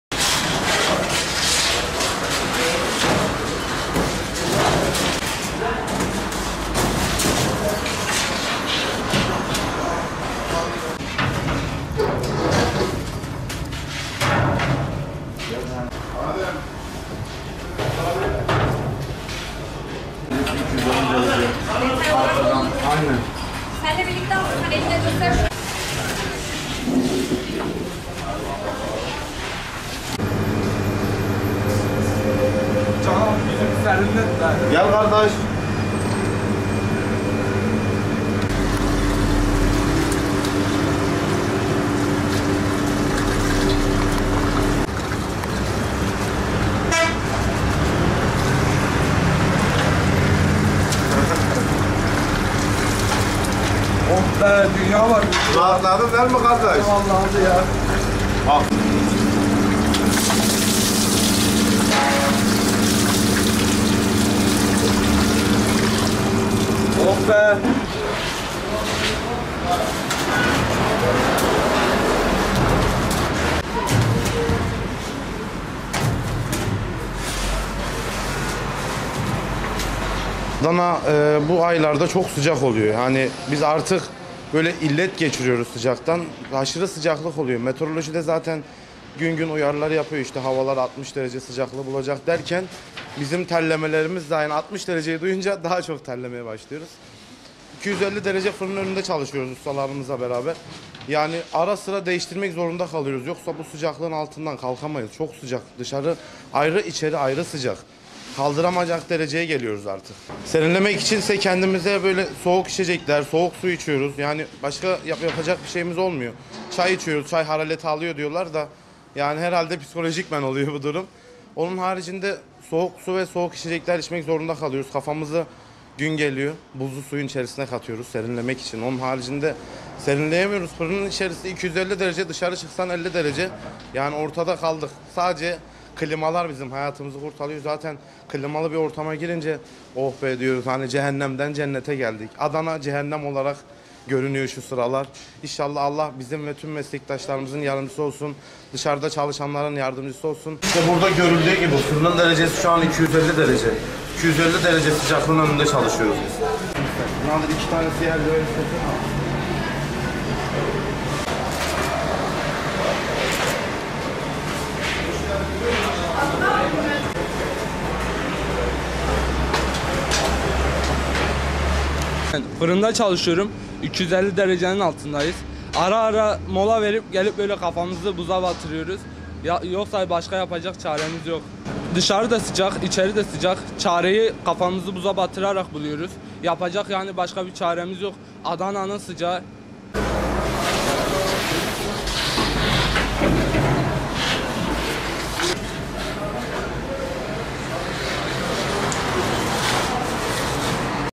Tam 250 derece! Adana'da fırıncılar kafalarını buzlu suya sokarak serinliyor
Türkiye’nin en sıcak illerinden Adana’da ‘El Nino’ sıcakları etkisini gösterirken, 250 dereceye ulaşan fırında ekmek pişiren ustalar kafalarını buzlu suya sokup hortumla serinlemeye çalıştı.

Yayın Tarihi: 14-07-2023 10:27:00